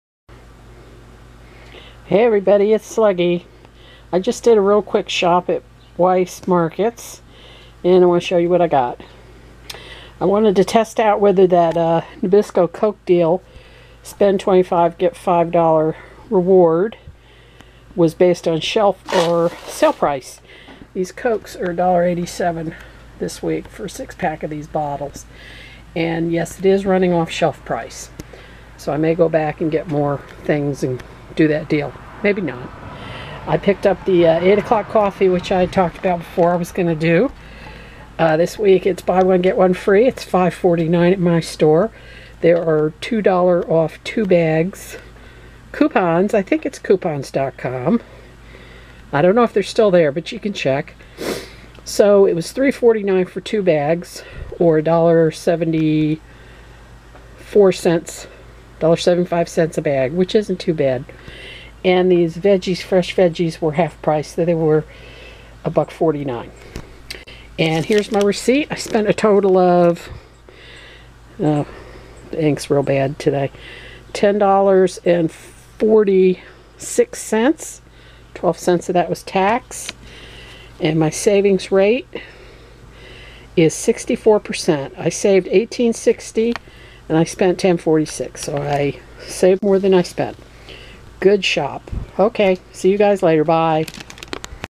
10 June 2015 Shopping
A quick trip to Weis(PMITA)Markets for a few items.
Tested the Coke/Nabisco Deal shelf vs. sale price.